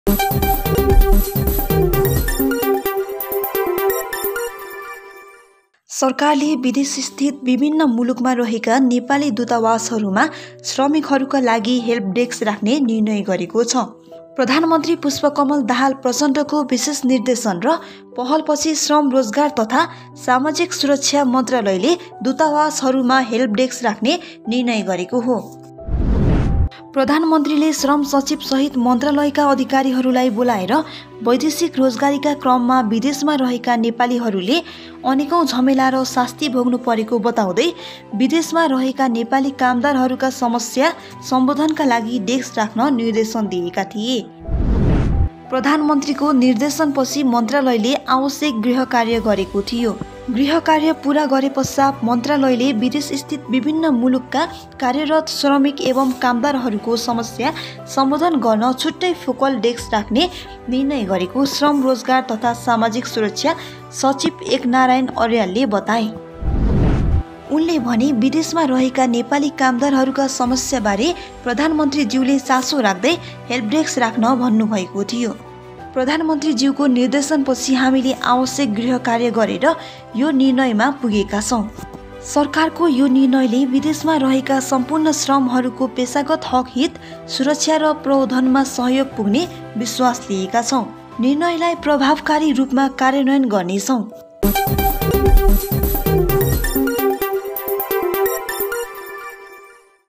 सबै नेपाली दुतावासमा 'श्रकिक हेल्प डेस्क' राख्ने सरकारको निर्णय
Guest:
Presenter: SRP
Camera: Sangit Nepali
Edit: Upesh Koirala
        Sangit Nepali

!...Unauthorized downloading and uploading on YouTube channel is Strictly Prohibited And will attract punitive measures from Micro Tv Nepal